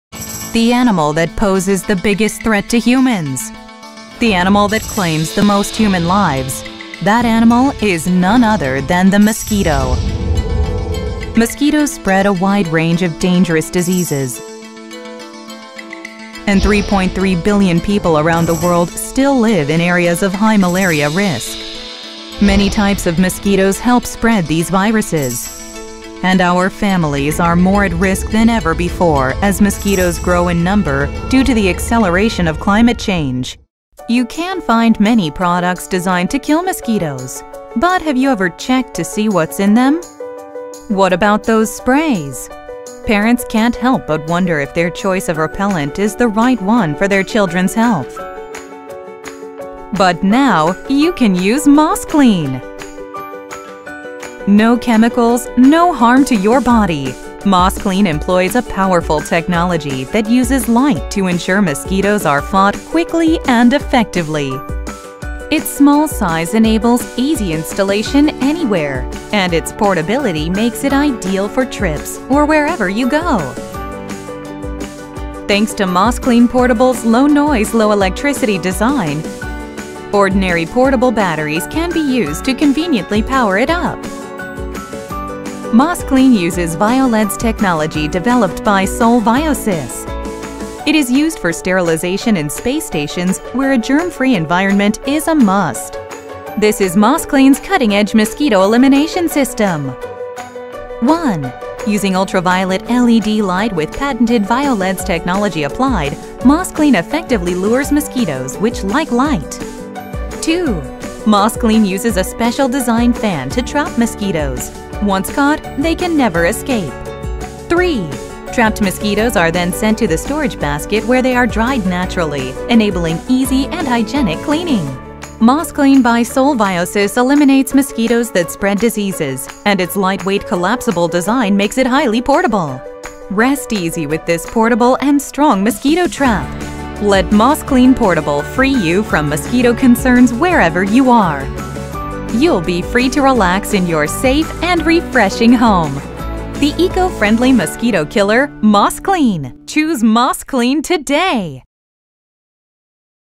[Violeds] 포충기 / Insect Trap
MOSCLEAN IP1 is the eco-friendly mosquito trap lures and traps mosquitoes by emitting an optimized UV LED light known to lure mosquites efficiently, using violeds technology. It performs up to 13times more effectively than existing mercury lamp insect traps in trapping of aedes aegypti, which is known to spread the Zika virus.
(Based on the test by the insect research center, University of Florida, U. S.)
MOSCLEAN IP1 is designed to be light and compact, so it is easy to use when traveling and camping.